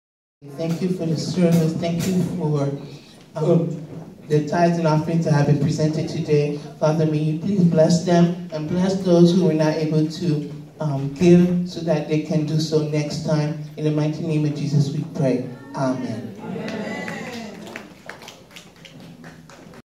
18 Prières pour les offrandes Diaco Charlotte MFCI Church Culte du 05 Nov 2017
MFCI Church - Culte du 05 Nov 2017
Thème du culte: CHERCHEZ PREMIÈREMENT LE ROYAUME DE DIEU | Pasteur Edmond Pierre
Visitez Nous:
. GOOGLE+: goo.gl/15gjW7